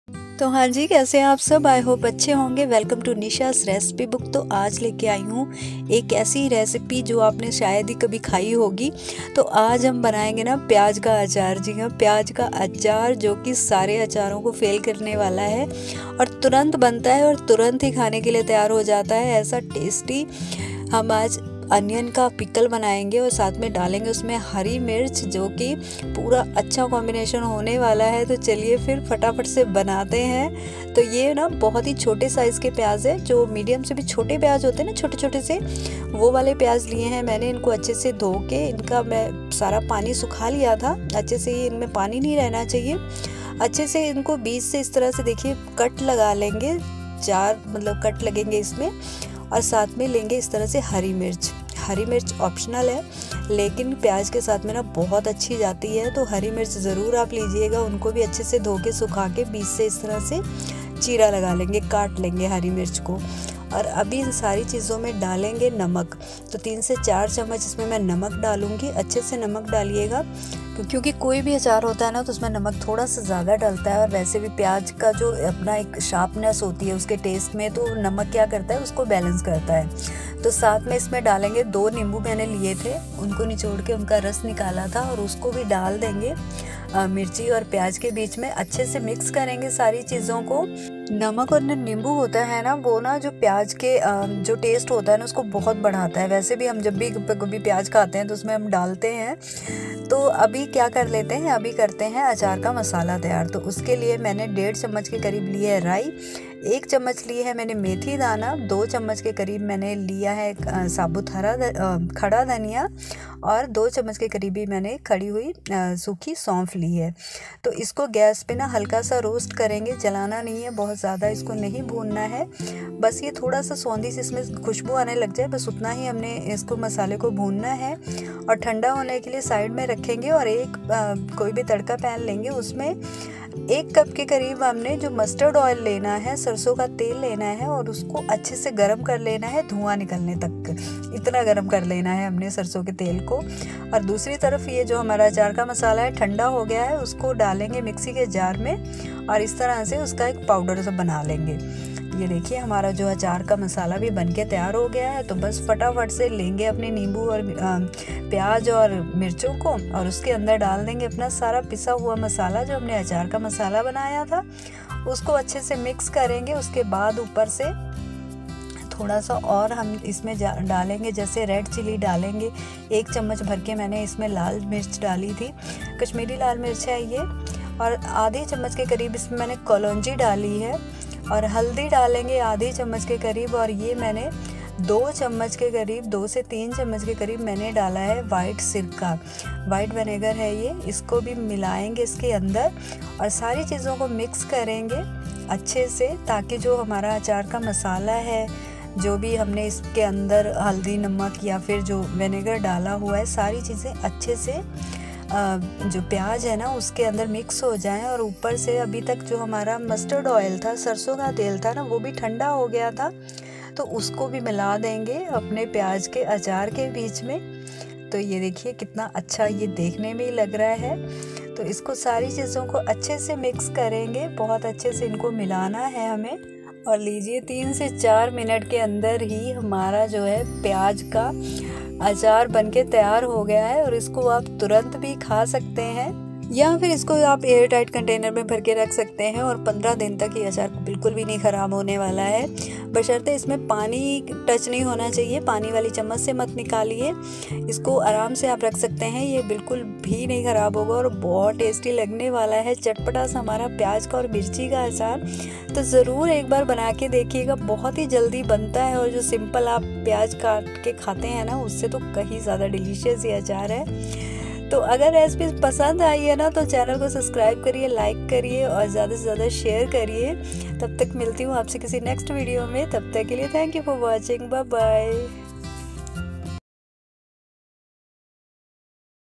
बस 5 मिनट में तैयार होगा यह चटपटा प्याज का अचार जो सारे अचार को को फेल कर देगा !!onion pickle
pickle onion Chilli pickle#instant pickle#pyaj Ka Achar#nisha's recipe book